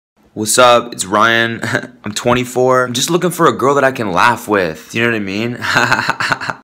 Animating Vines: If Tinder had video profiles
A vine by Cody Ko, animated by Christopher Pridnia.